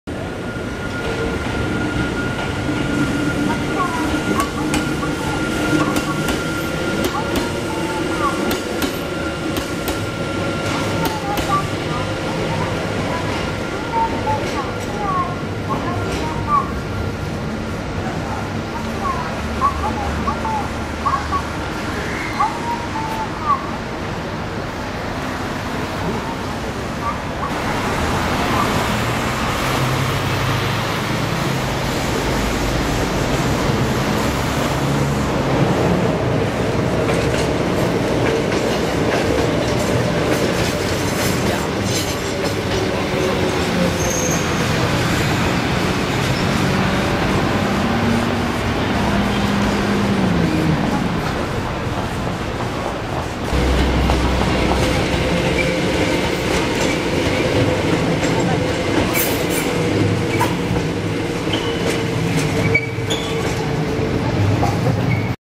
箱根登山鉄道 モハ1形・モハ2形・1000形電車 走行 箱根湯本駅 2011/12/23
粘着式鉄道としては日本で一番急な勾配を走行する箱根登山鉄道の電車。